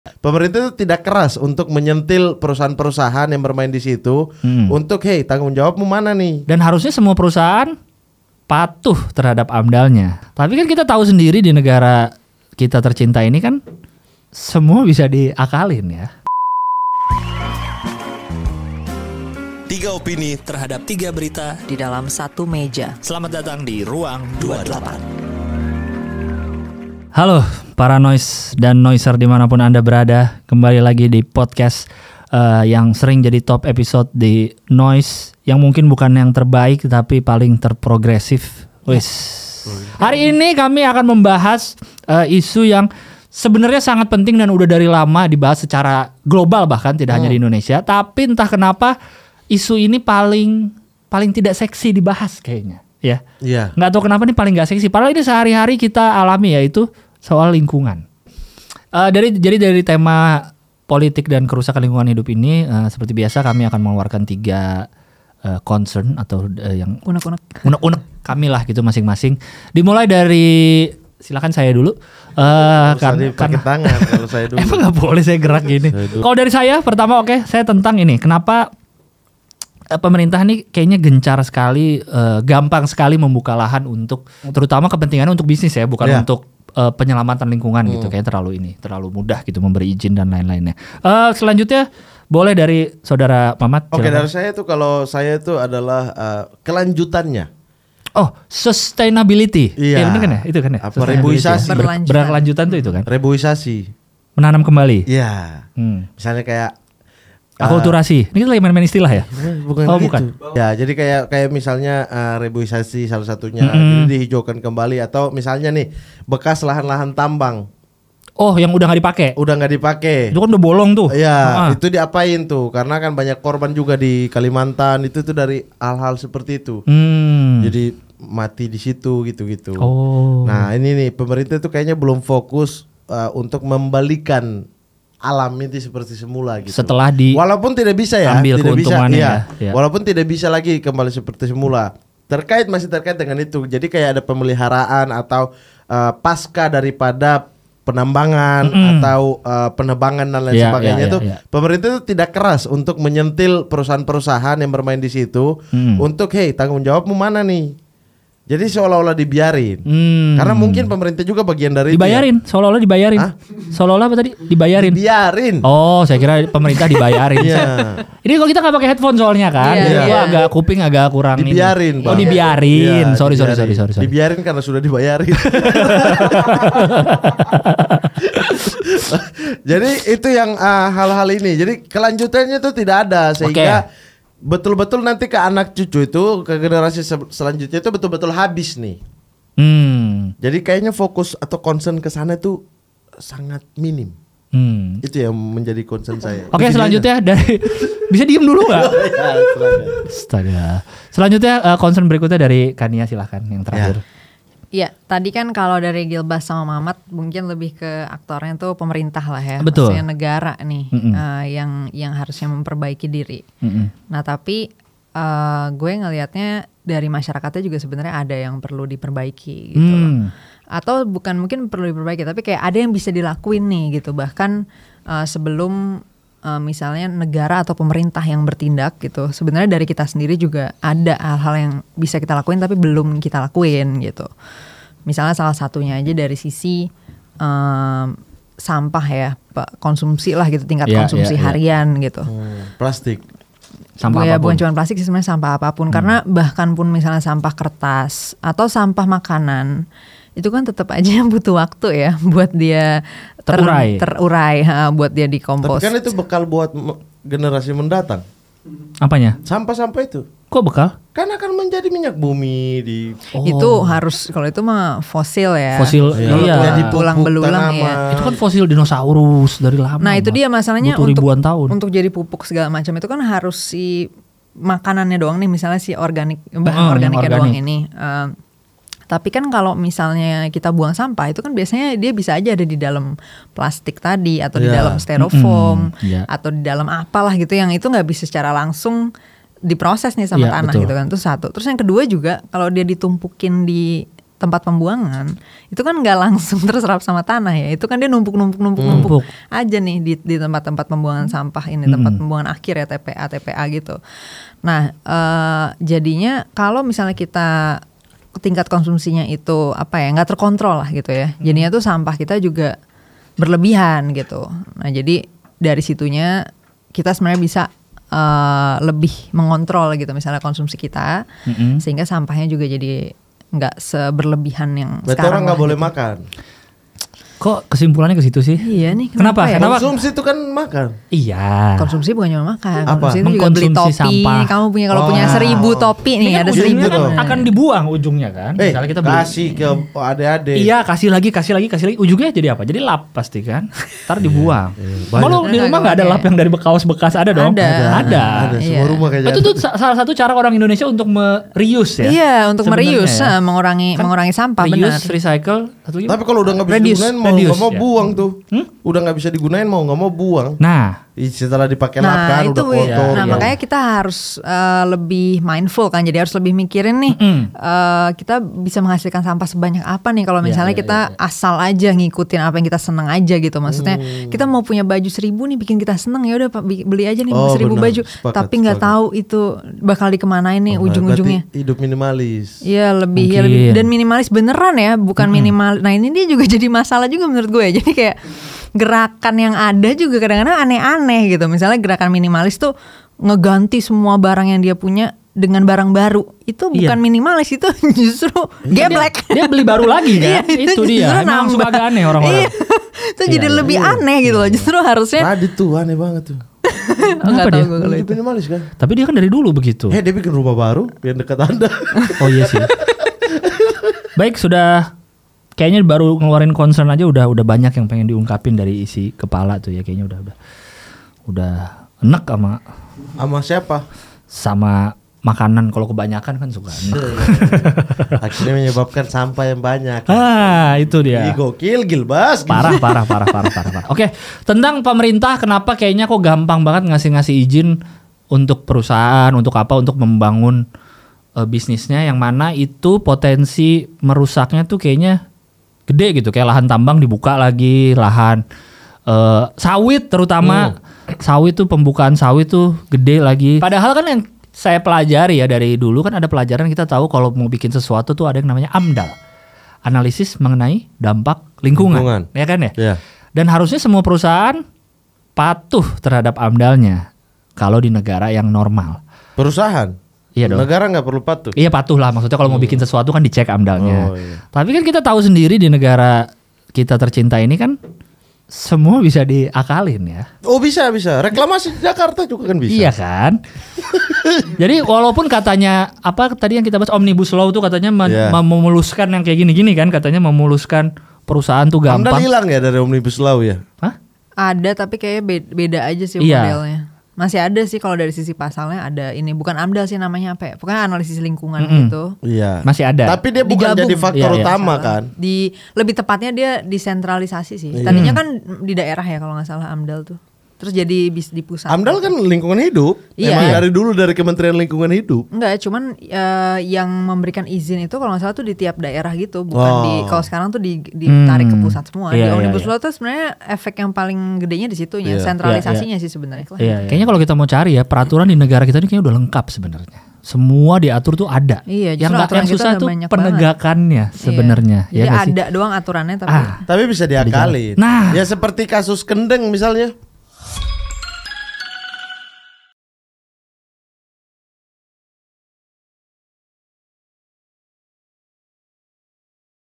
Mamat Percaya Kerusakan Alam Karena Politik Kotor di Indonesia!!! Ruang 28 Mamat, Cania, Gilang
Mamat, Cania dan Gilang dibikin bingung kenapa alam di Indonesia gampang banget di eksploitasi??Pelolosan izin, politik-politik kotor, kerjasama terselubung untuk para pembisnis yang merusak lingkungan semakin mengkhawatirkan di Indonesia, mencari keuntungan tanpa diimbangi kesadaran lingkungan, dan revitalisasi yang minim membuat kondisi alam semakin sulit untuk di tolong. Selengkapnya hanya di

0:00 Isu Kerusakan Lingkungan di Indonesia
3:21 Gak Melulu Salah Pemerintah!!
7:14 Pemerintah Terlalu Gampang Kasih Ijin